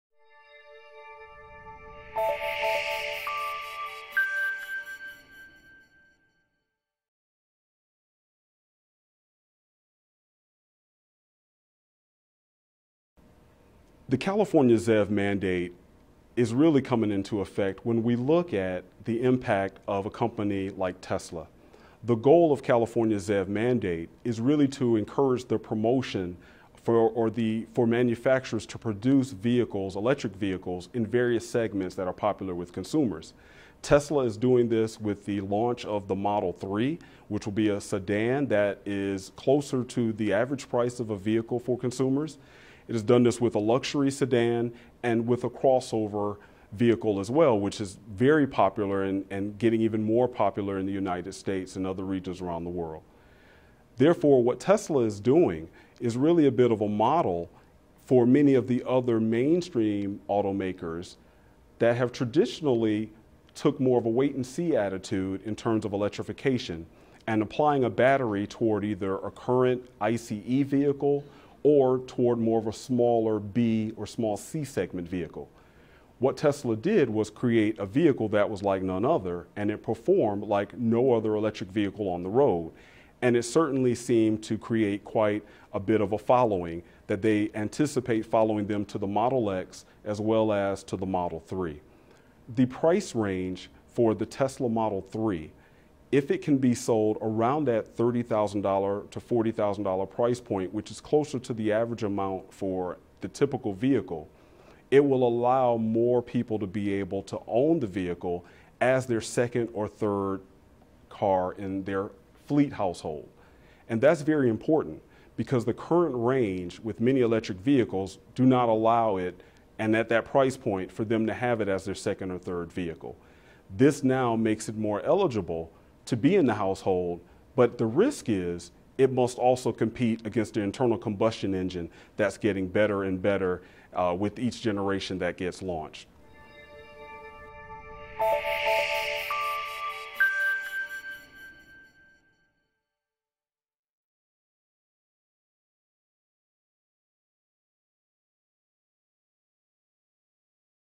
Future Impacts of the California Zero Emission Vehicle (ZEV) Mandate
IHS Automotive principal analyst Devin Lindsay talks about the impact Tesla will have on California's ZEV mandate and how both will effect the state of the automotive manufacturing industry